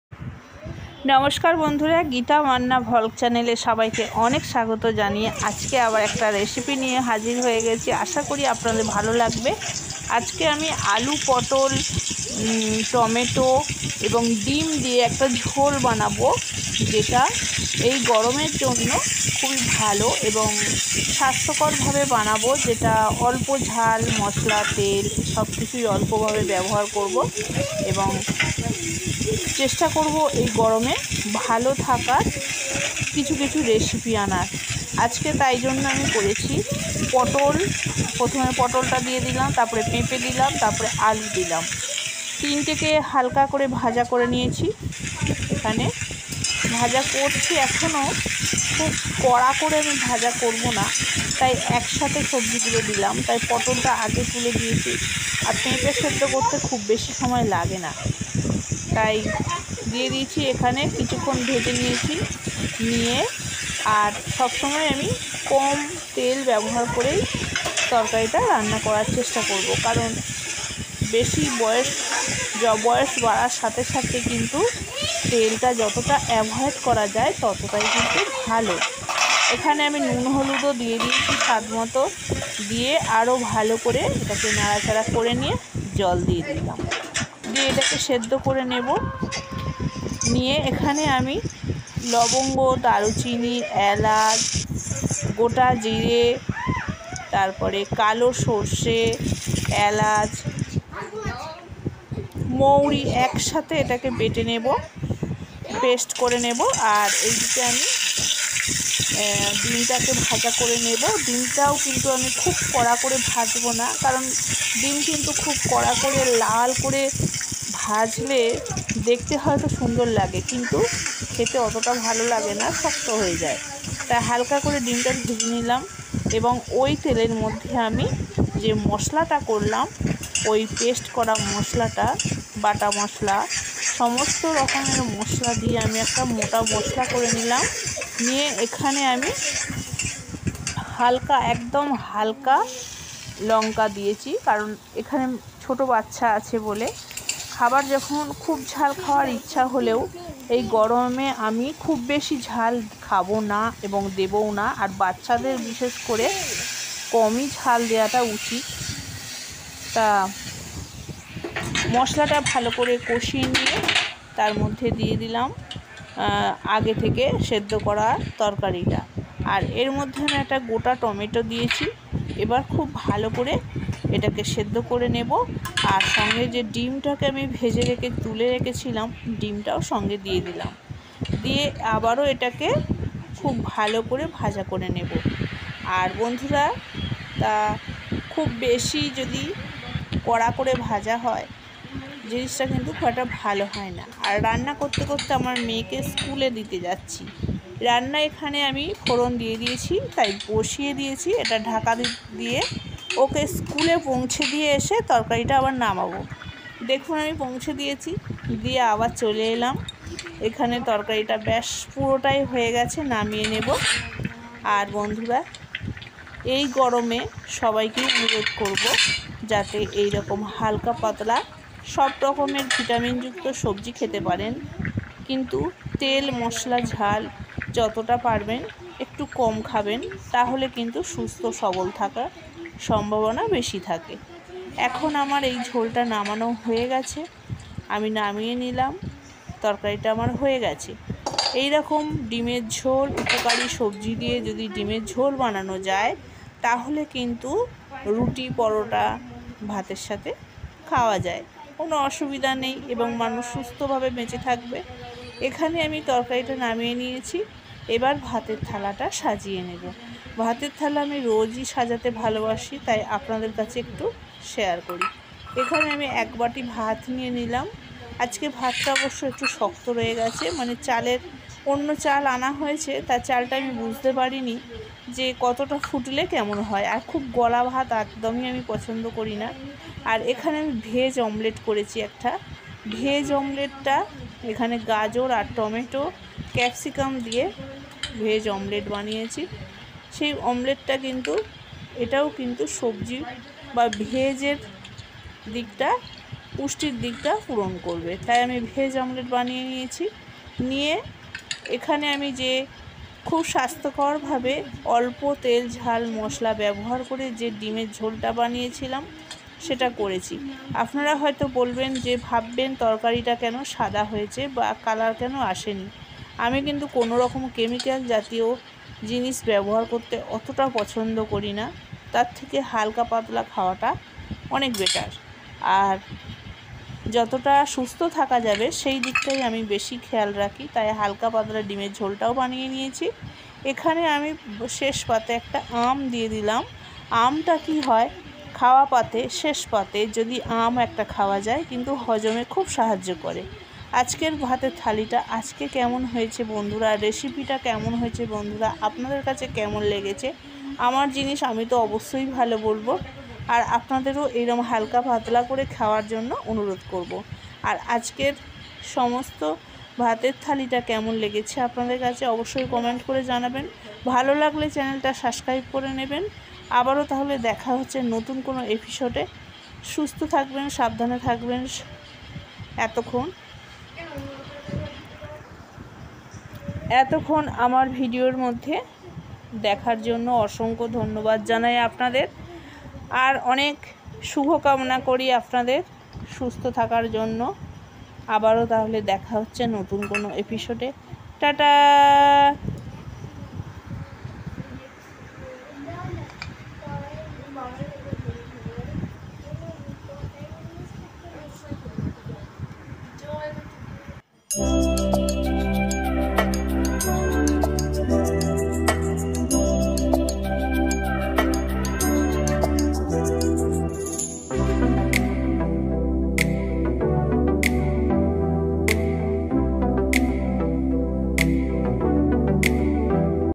Egg vegetables recipe
Egg vegetables recipe gitamannavlog youtube video
আশা করি বন্ধুরা ভালো আছেন।হালকা পাতলা ঝোশ স্থাস্থ্যের পক্ষে ভালো তাই সুস্থ্ থাকার অন্য আপনাদের অনুরোধ রাখলাম এই রকম ঝোল খাওয়ার।
সব মশলা নিয়েছি হাফ চামচ করে
তিনশ পটল
একটা খড় গোটা আলু
একটা মাঝারি পেপে।